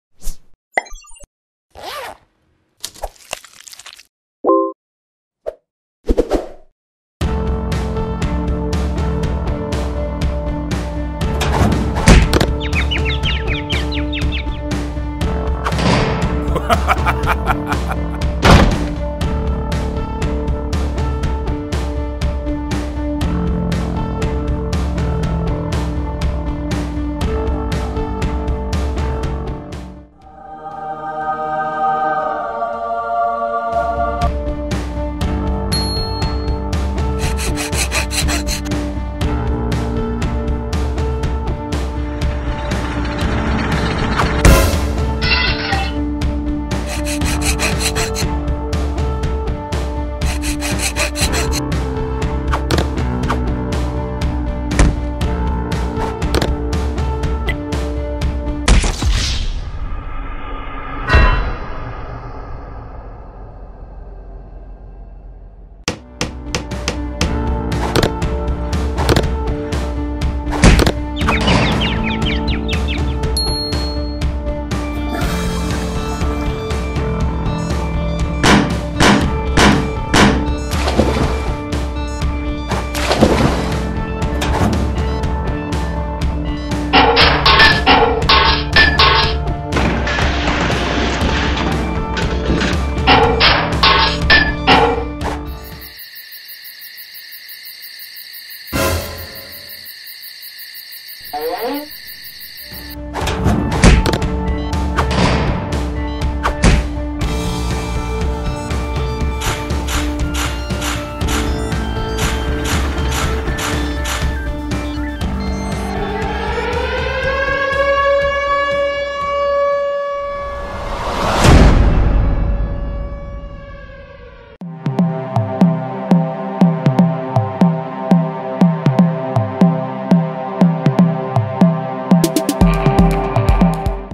Побег - Мультики про танки
Мультик про танки по мотивам игры Armored Warfare.
Никогда не сдавайся!

►Не забывайте оценить мультик Armored Warfare и оставить свои комментарии! Мне будет очень приятно ощутить вашу поддержку!
►Новые серии в конце каждой недели

Анимация создана в программе Adobe Flash Pro